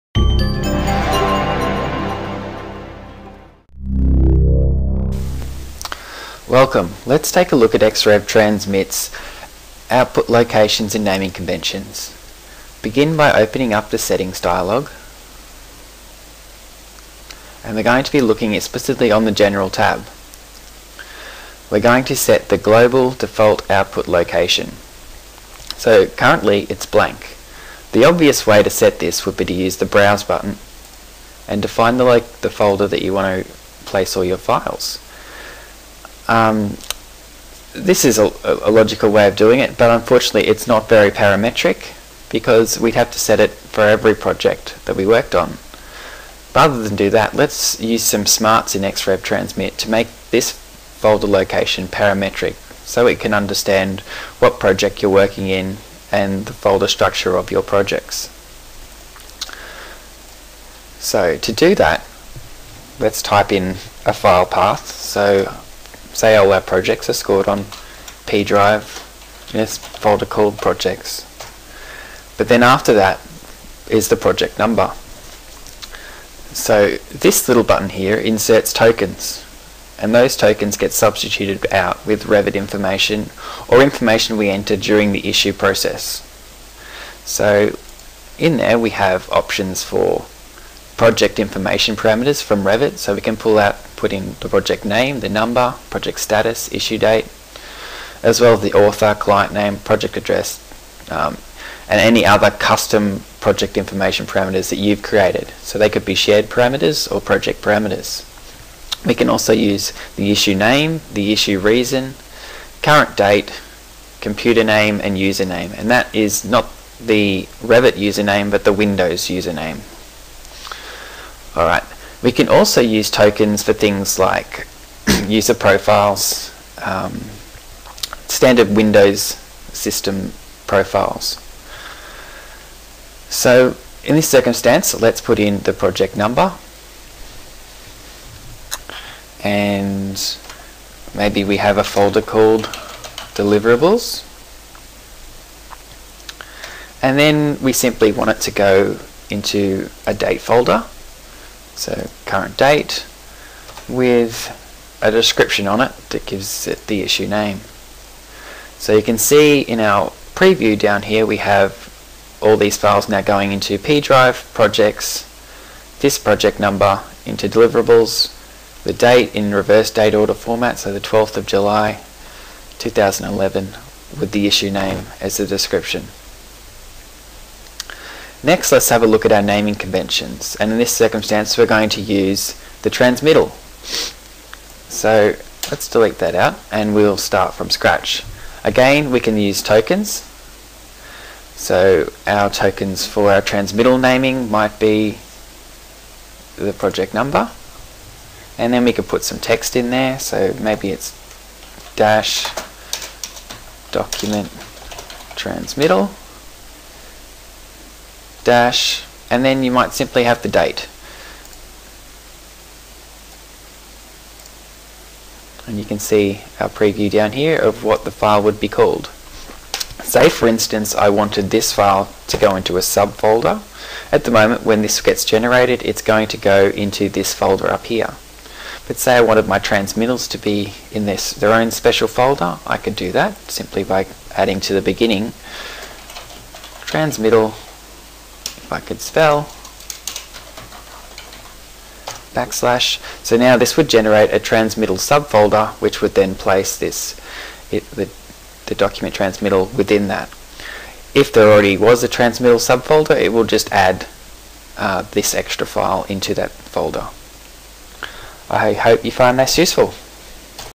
Xrev Transmit - Setting a Naming Convention and Output Paths
This video demonstrates how to setup the Output Locations and Naming Conventions.  It details how to use the Revit Parameter tokens and the various advanced parametric abilities that are possible to save you time and money.